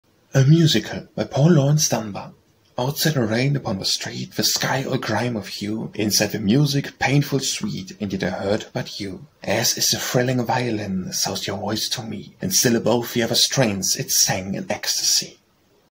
A Musical - Paul Laurence Dunbar
A Musical
Paul Laurence Dunbar

    Outside the rain upon the street,
    The sky all grim of hue,
    Inside, the music-painful sweet,
    And yet I heard but you.

    As is a thrilling violin,
    So is your voice to me,
    And still above the other strains,
    It sang in ecstasy.